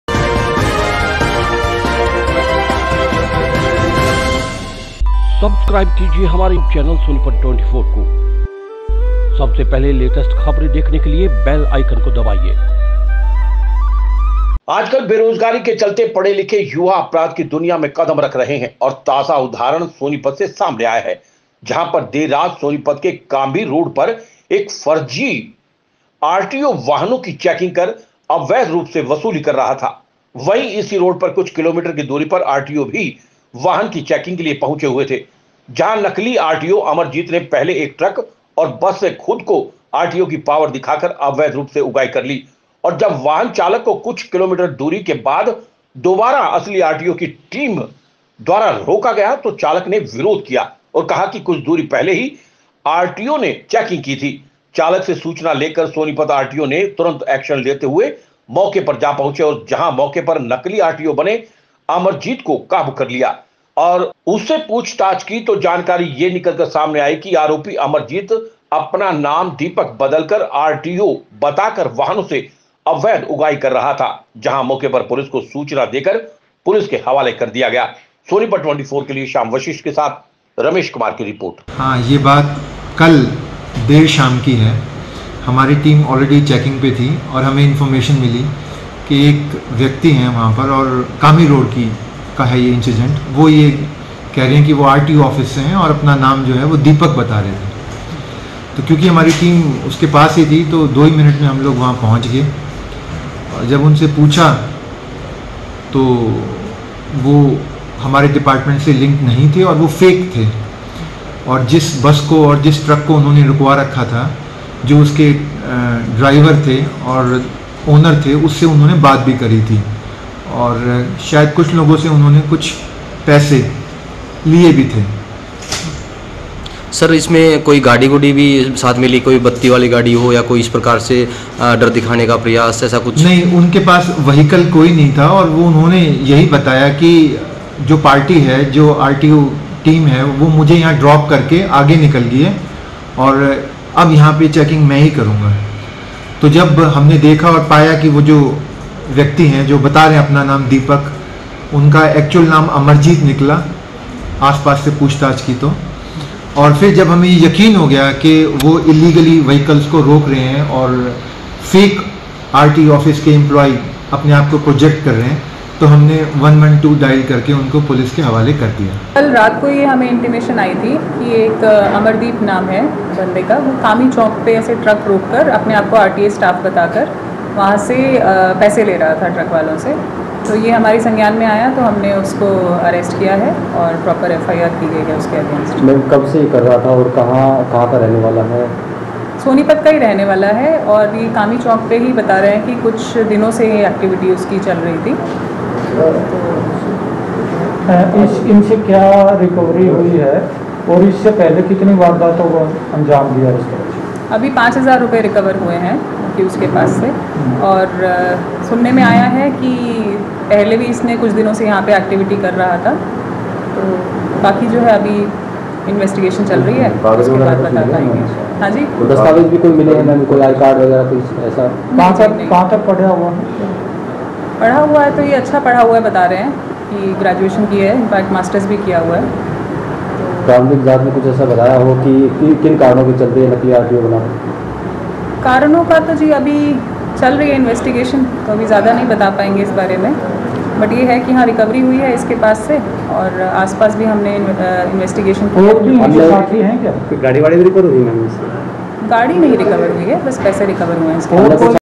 R T O बन करता था वसूली, चढ़ा पुलिस के हत्थे || Sonipat24
वाहन चालक सावधान सड़क पर नकली आरटीओ करते हैं चेकिंग

सोनीपत के कामी रोड से गिरफ्तार हुआ नकली आरटीओ

ट्रक चालकों से करता था अवैध वसूली

सदर थाना पुलिस कर रही मामले में जांच

सोनीपत में वाहनों से अवैध वसूली करने वाला फर्जी आरटीओ गिरफ्तार हुआ है। गिरफ्तार आरोपी अमरजीत सोनीपत का रहने वाला है।देर रात सोनीपत आरटीओ अपनी टीम के साथ वाहनों का चेकिंग अभियान चलाए हुए थे और जहां एक वाहन चालक के  दो जगह  रोके जाने पर आपत्ति जताई तो पूरे मसले का खुलासा हुआ। देखिए नकली आरटीओ की अवैध वसूली का कैसे खुलासा हुआ।इस खास रिपोर्ट में देखिये।